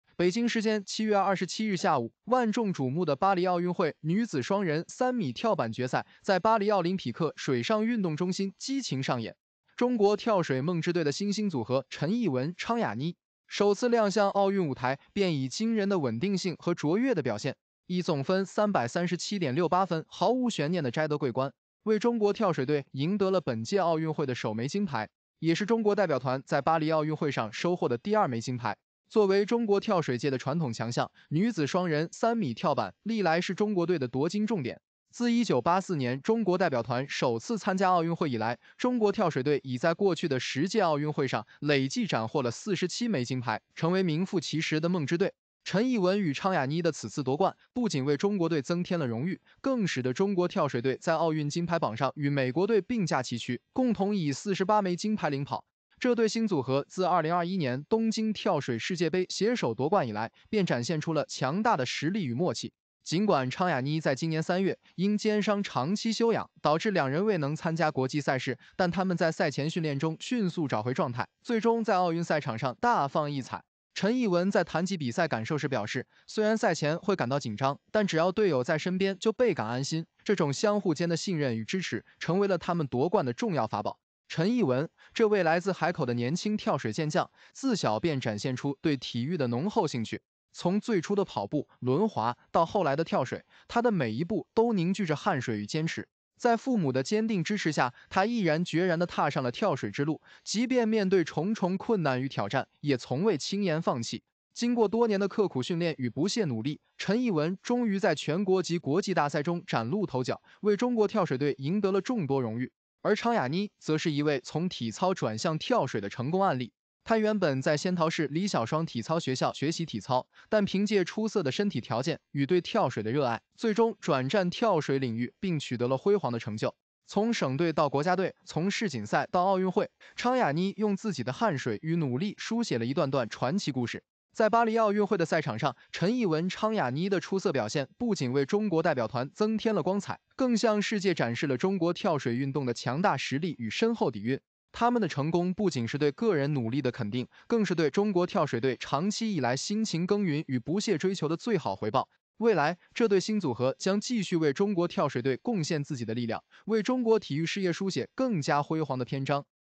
第二金！陈艺文/昌雅妮夺得跳水女子双人三米板金牌，巴黎奥运会中国队与美国队48金并列第一
毫无悬念！陈艺文/昌雅妮夺得跳水女子双人三米板金牌